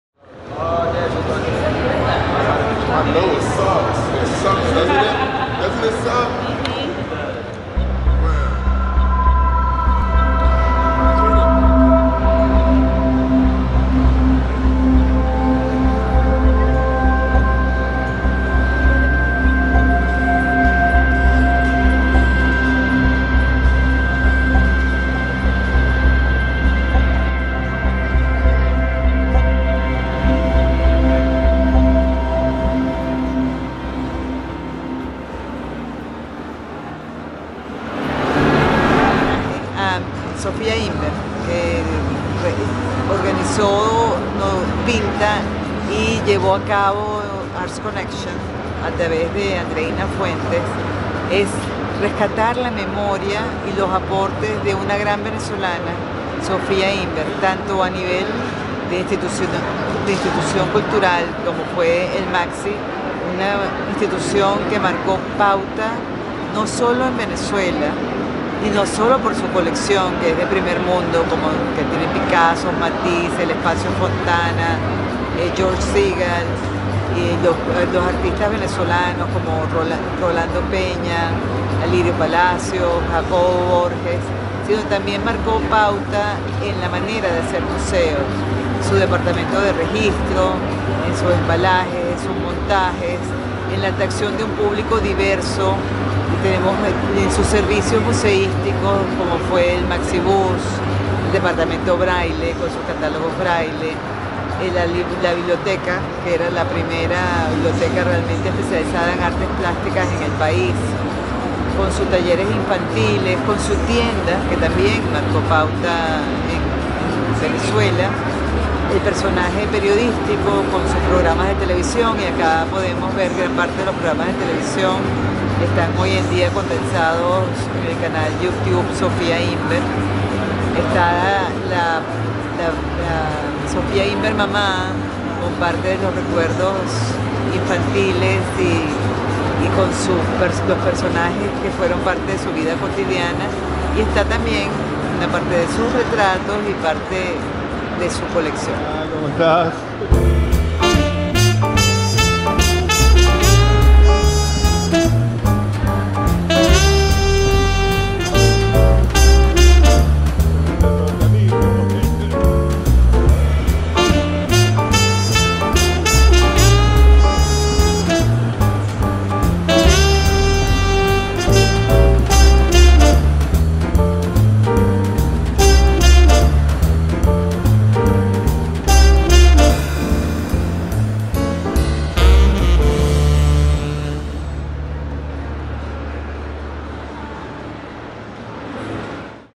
Pinta Presents: TRIBUTE TO SOFÍA IMBER
Sofía Imber, (Rumania, 1924 – Caracas, 2017)
Journalist, Art Critic and Museum Director.
The decisive participation of Sofía Ímber in the development of journalism, art criticism and Venezuelan cultural institutions make her one of the most influential figures in the contemporary history of the country and Latin America. With an attractive personality, inimitably controversial style, radical and exemplary commitment to work and critical acuity when interpreting creative and social processes, she carried out the necessary transformations to bring the country's museums to a level of quality marked by excellence in the formation of collections and communicational and educational activities.

The pioneering work of Sofia allowed to open the way for Venezuelan women in fields previously not traveled. She was the first woman journalist, the first director of a section of Cultural Pages in the press, first host of an opinion program on television and the first director of a museum in Venezuela. Through the creation and direction of her main accomplishment, the Museum of Contemporary Art of Caracas Sofia Imber, in 1974, she changed the methods of conducting museums and cultural institutions in the country, stressing the importance of the quality of exhibitions, the achievement of excellence, the formation of an unrivaled collection, education programs and the overall communicative, innovative and integrating spirit of the MACCSI. Her contributions are not limited to the journalistic and cultural spectrum, but Sofia Imber is an example in the defense of the essential values ​​of the definition of the human being and work for freedom and democracy.